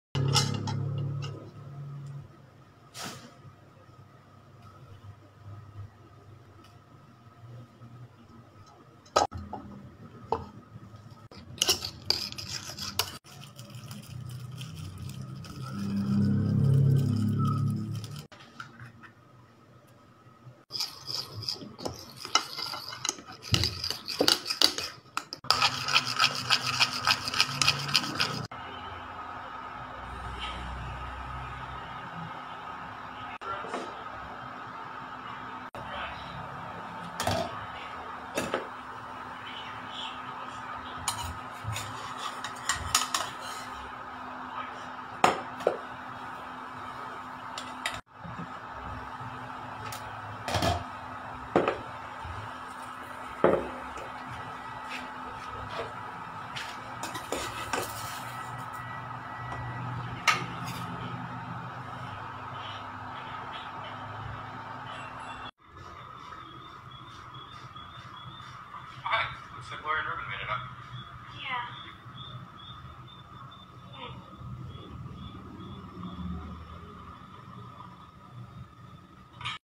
How to make banana pancakes without oven
homemade snacks/merienda

ingredients:
banana
flour
egg
water
no need of sugar just original flavor of banana